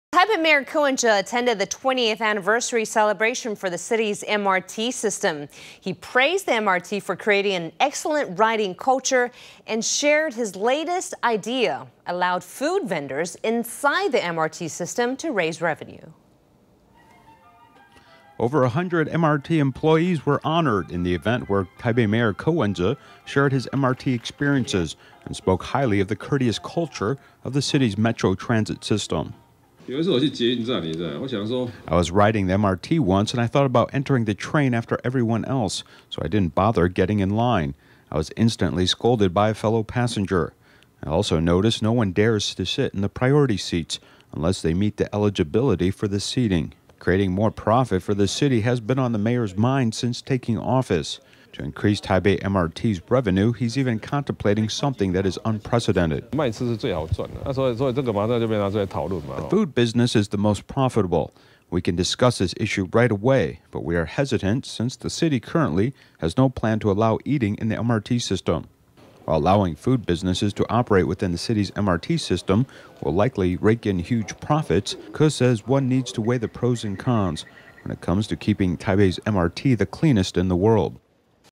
Taipei Mayor Ko Wen-je praises MRT, considers allowing food vending in MRT
Taipei Mayor Ko Wen-je attended the 20th anniversary celebration for the city’s MRT system. He praised the MRT for creating an excellent riding culture and shared his latest idea— allow food vendors inside the MRT system to raise revenue.Over a hundred MRT employees were honored in the event where Taipei Mayor Ko Wen-je shared his MRT experiences and spoke highly of the courteous culture of the city’s metro transit system.Ko Wen-jeTaipei Mayor I was riding the MRT once and I thought about entering the train after everyone else so I didn’t both getting in line. I was instantly scolded by a fellow passenger. I also notice no one dares to sit in the priority seats unless they meet the eligibility for this seating.Creating more profit for the city has been on the mayor’s mind since taking office. To increase Taipei MRT’s revenue, he’s even contemplating something that is unprecedented.Ko Wen-jeTaipei Mayor The food business is the most profitable. We can discuss this issue right away but we are hesitant since the city currently has no plan to allow eating in the MRT system.While allowing food businesses to operate within the city’s MRT system will likely rake in huge profits, Ko says one needs to weigh the pros and cons when it comes to keeping Taipei’s MRT the cleanest in the world.